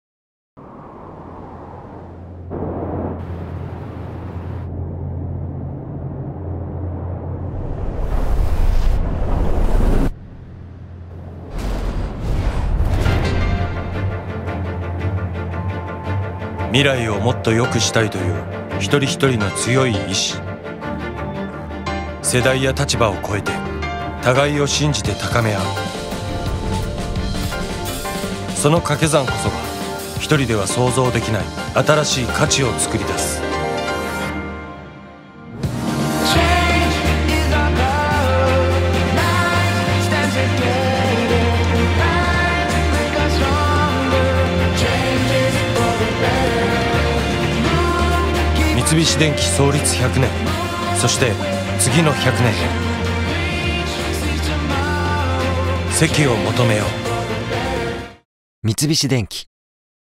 三菱電機100周年「積を求めよう。」
100周年を迎えた三菱電機のTVCMを制作。
「積を求めよう。」というコピーを元に、社員一人一人の個性を掛け合わせた先にある未知の可能性を表現した。長い歴史を持つ三菱電機ならではの重厚感と、鮮やかで個性的な未来への期待感を融合させ、印象的な作品に仕上げた。

WOW produced a television commercial for Mitsubishi Electric, which celebrated its 100th anniversary.
Based upon the catch copy, “Create new value together,” WOW expressed the unknown possibilities that exist in a future in which the individuality of each of the company's employees is multiplied. WOW fused the profound feelings that uniquely apply to Mitsubishi Electric with its long history and the feelings of expectation for a bright and individualistic future. WOW then finalized it all into an impressive work.

Credits
Creative Director ： Kousuke Oho
Director： Ryo Kitabatake
CG Designer ： Taishi Yagyu (dep MANAGEMENT)、Takuma Sasaki、Ryouichi Kuboike
Producer：Go Hagiwara

Agency ： ORANGE AND PARTNERS
Director of Photography ： Nobuaki Imura (IMURA OFFICE)
Lighting & Gaffer ： Koji Yoshino (LightNix inc.)
Volumetric ： Yasunori Aoki (Sony)
Matchmove Artist ： Yasuhiro Kato
Hair & Make-up Artist ： Rie Aoki
Stylist ： Miki Nakamura
Casting Director ： Hisashi Shibuya (Kettle)
Location Coordinator ： Hajime Nakagaki (Groove)
Color Grading ： Haruna Okutsu (IMURA OFFICE)
Off-line Editor : Fumihito Endo (jitto)
On-line Editor : Akio Sakamaki (jitto)
Mixer ： Katsuyuki Yamada (Onkyo House)
Production Manager ： Sanchu, Naoki Atarashi
WOW inc.